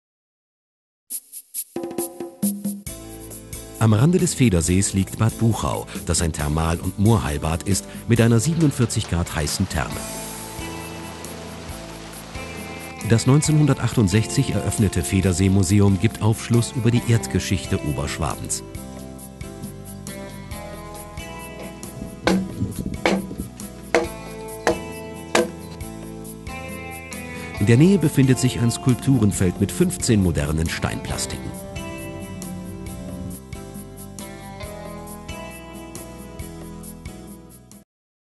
Bodensee Bad Buchau Videoportal - BodenseeTV.TV
Ortsportrait über Bad Buchau Bodensee, produziert von Freizeit TV Bodensee. Hier erfahren Sie alles Wichtige rund um die Stadt und erhalten einen Überblick über Kultur, Freizeitmöglichkeiten und Sehenswürdigkeiten.